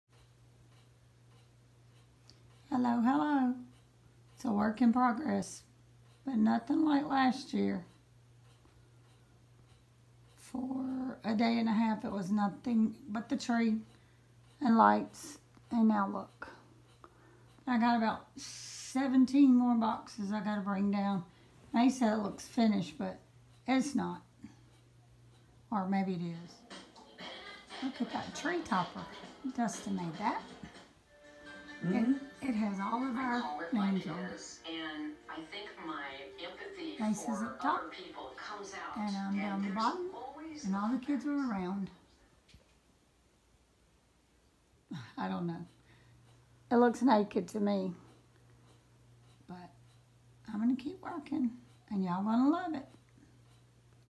Working on the tree
Getting the tree decorated a little at a time.